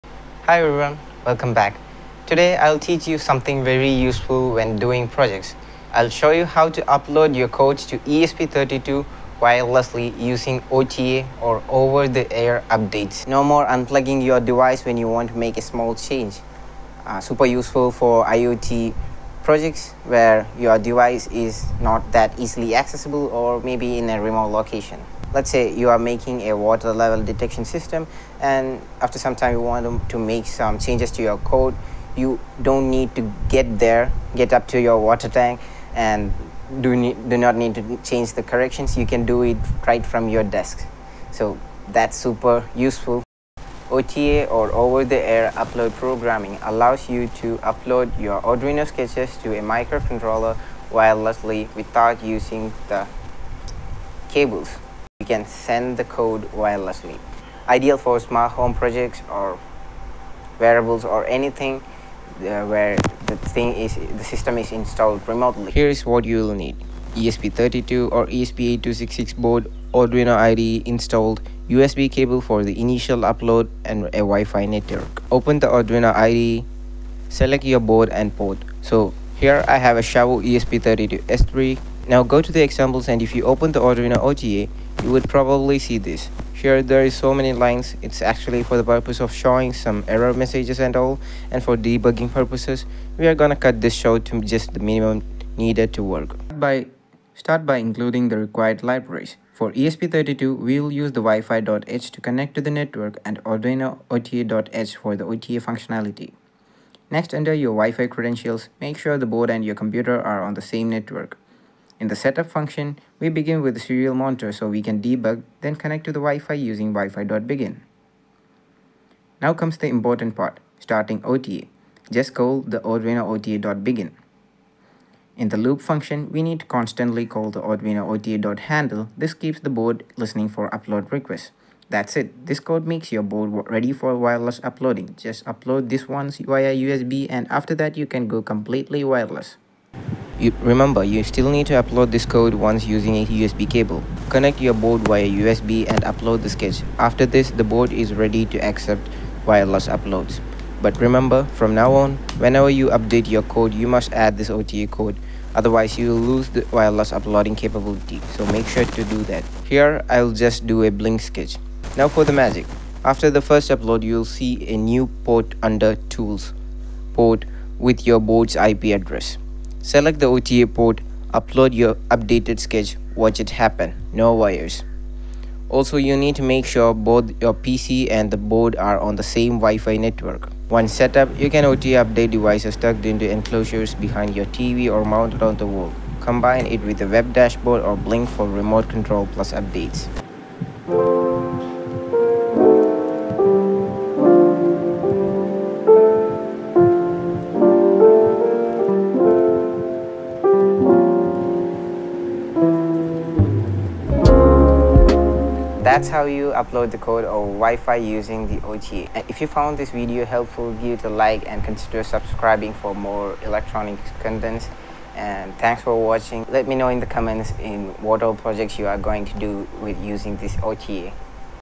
How to Upload Code Over Wi-Fi to ESP Boards (ESP32/ESP8266 OTA) | VinVout
In this tutorial, I’ll walk you through everything you need to know about setting up wireless code uploading using the Arduino IDE. Whether you're building an IoT project, a smart home device, or just want to make your workflow more efficient, OTA is a game changer!

👉 What you'll learn in this video:

What is OTA and why it's useful

Setting up OTA with ESP32/ESP8266

Writing and uploading OTA-compatible code

Uploading sketches wirelessly via Wi-Fi

Common issues and how to troubleshoot them